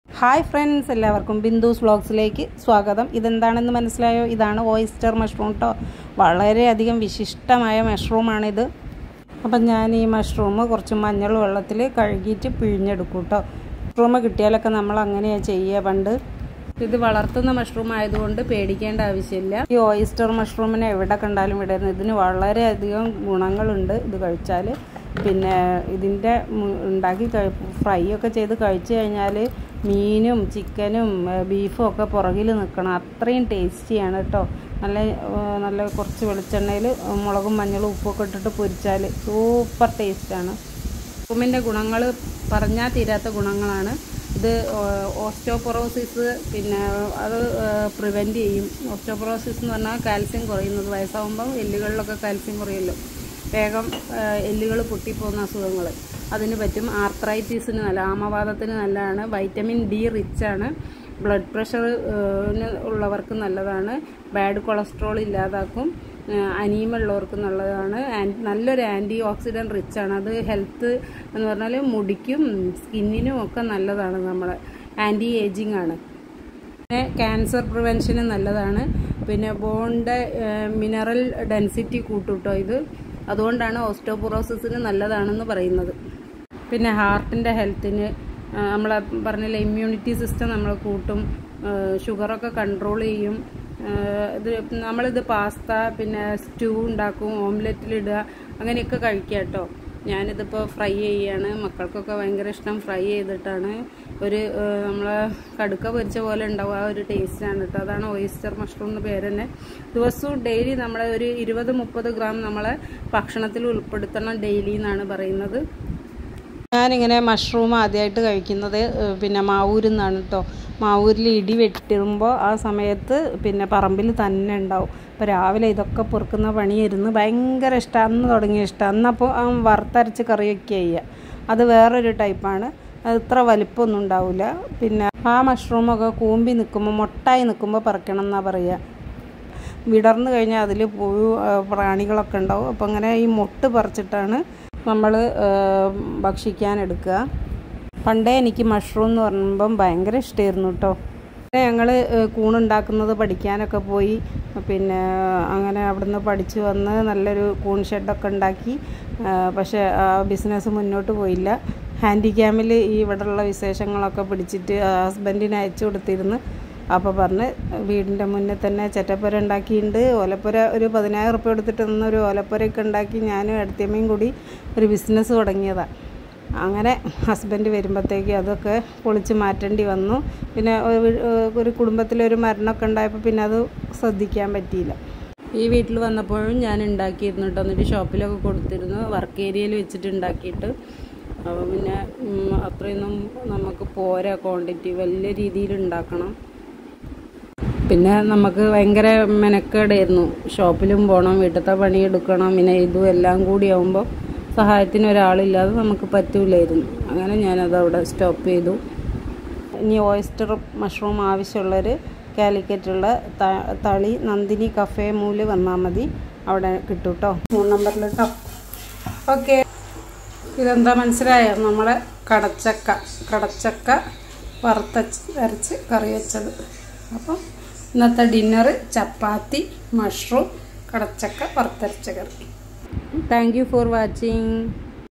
oystermushroom അഥവാ ചിപ്പിക്കൂൺ.ആരോഗ്യത്തിന്റെ കലവറ.
daily 20,30 gram ഭക്ഷണത്തിൽ ഉൾപ്പെടുത്തണം ആരോഗ്യം സർവ്വ ധനാൽ പ്രധാനം 👍🏻😍health